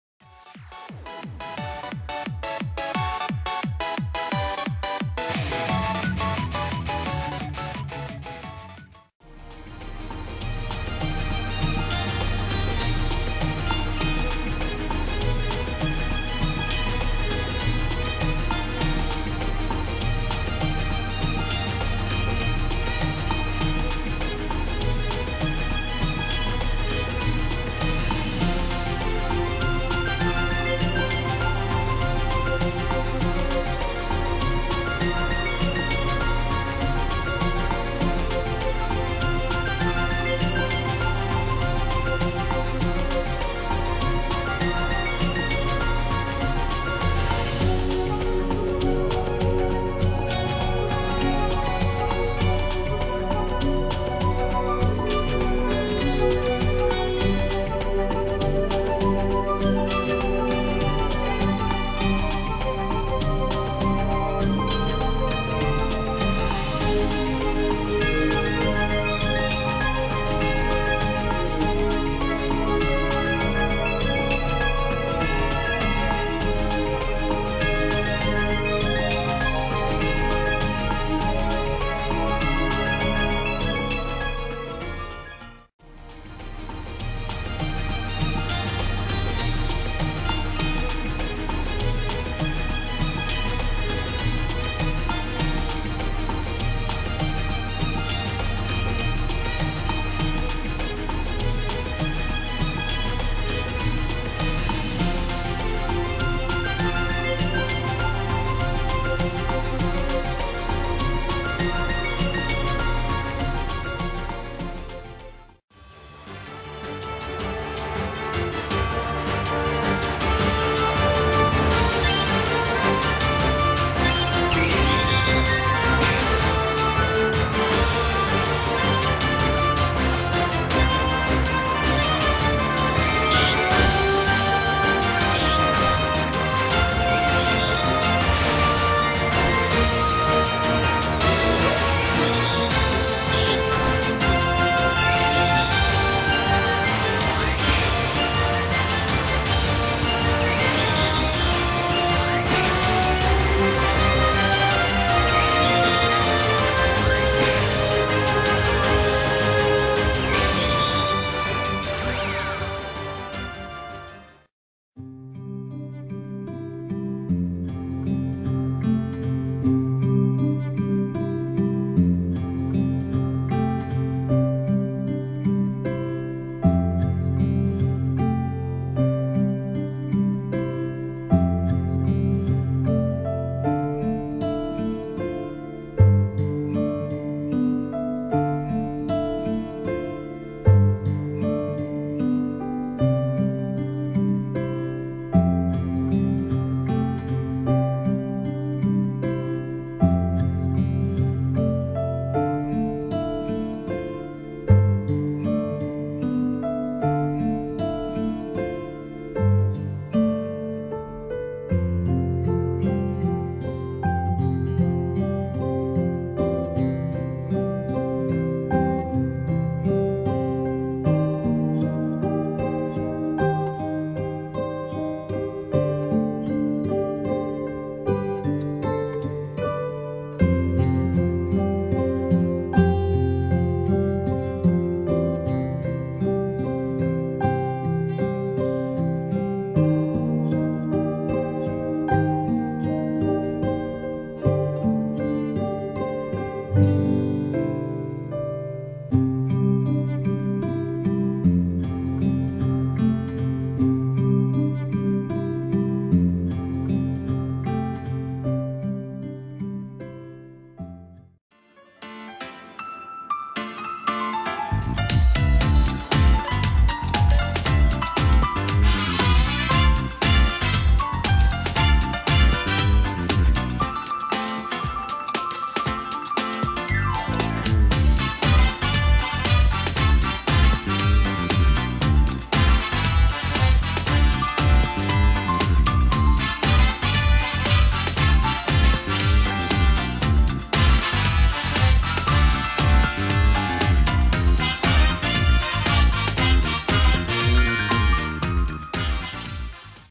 Tips & Tricks - Heal Jammer - RPG Maker MV
Heal Jammer is an effect that blocks up to a certain amount of direct healing. In this case, 3,000 HP will be blocked. Once the blocked values are used up, Heal Jammer will then remove itself.